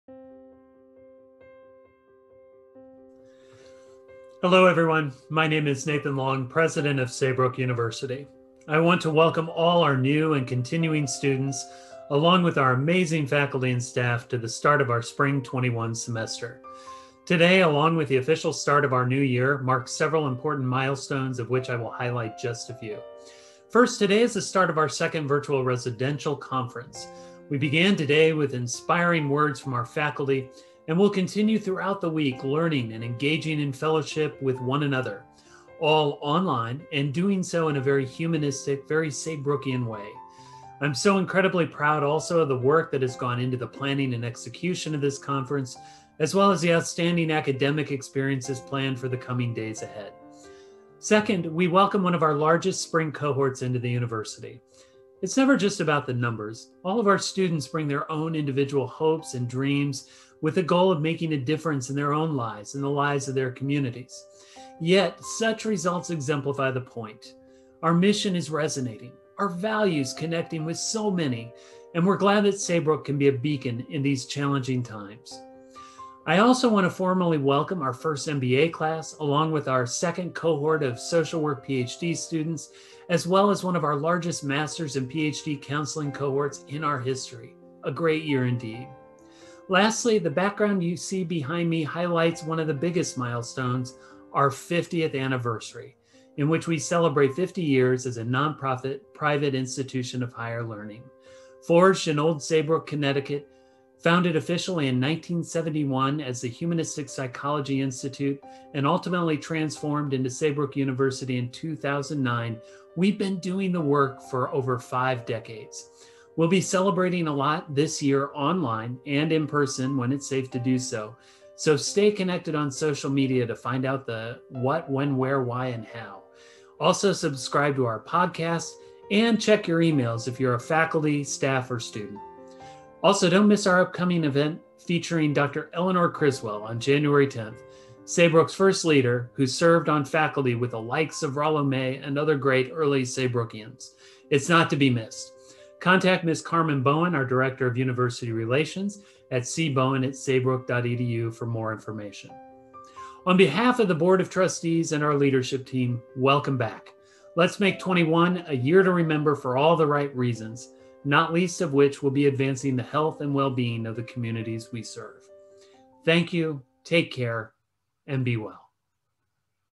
Saybrook U President'sWelcome 2021
A wonderful first day opening the Spring 2021 semester! Here is my short welcome video to our entire community highlighting some milestones for 2021. Have a great week, everyone. See you here and elsewhere online!